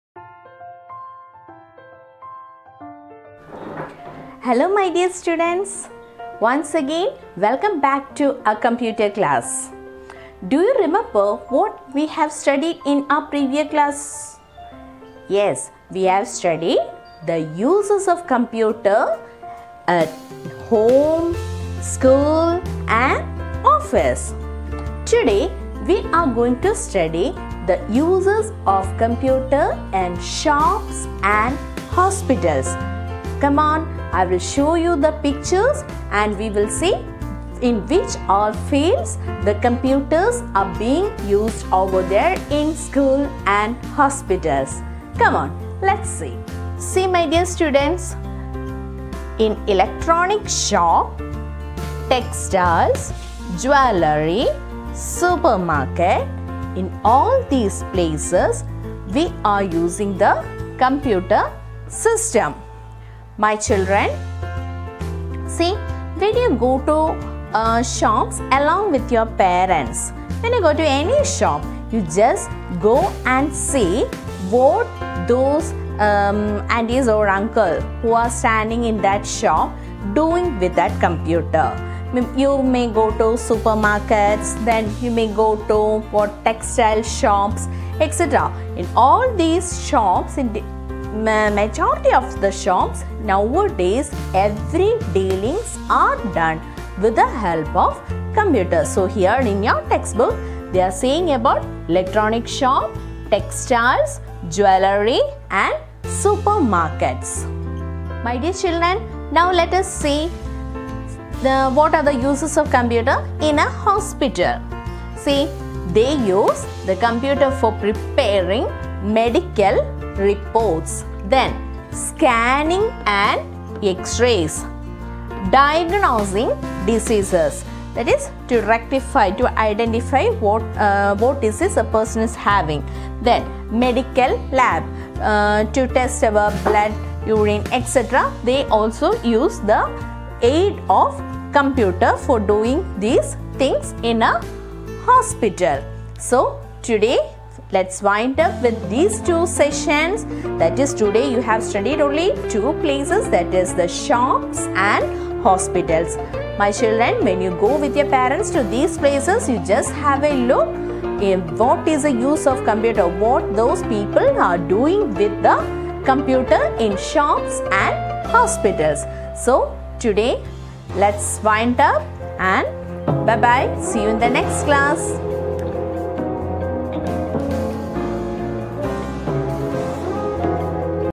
Computer on demand in shops and hospitals
Picture based teaching video on Uses of computer in shops and hospitals